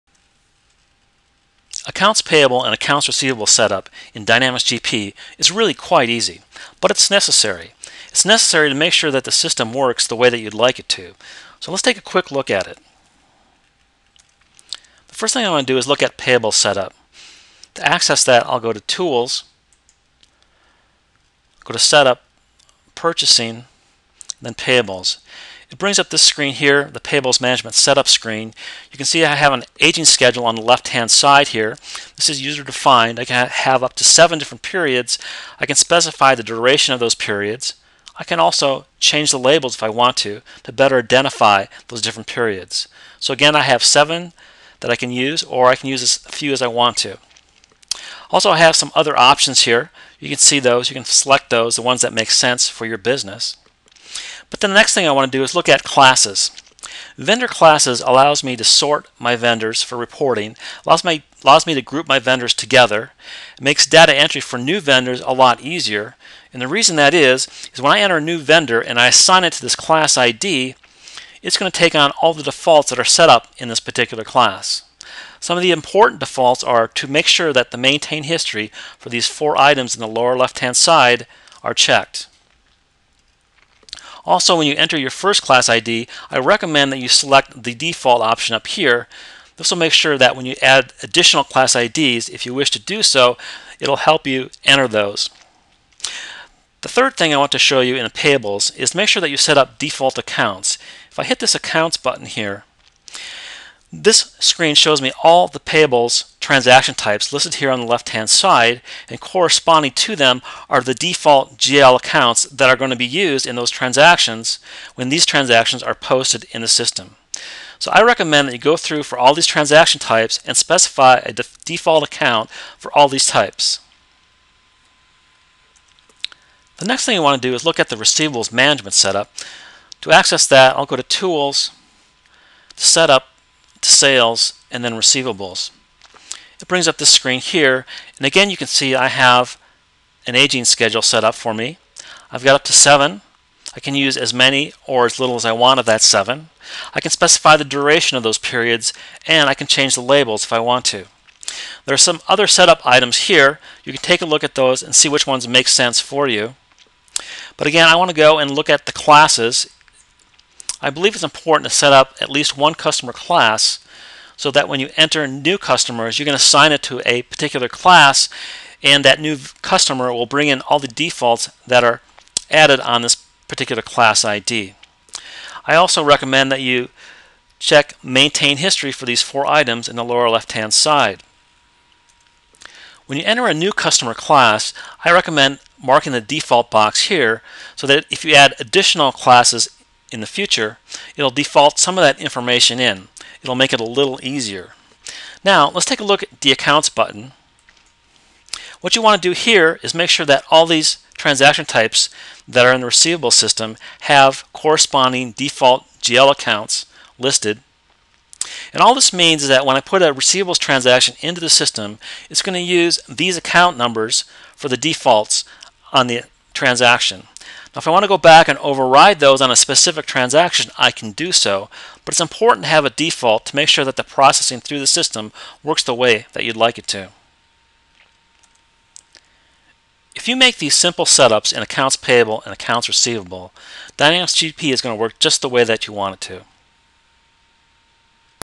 Accounts Payable and Accounts Receivables Setup in Dynamics GP
This video shows setups you can make in Accounts Payable and Accounts Receivables in Dynamics GP.  These simple setups will make the system work the way you'd like it to. This is among the first steps you'll take in setting up Dynamics GP.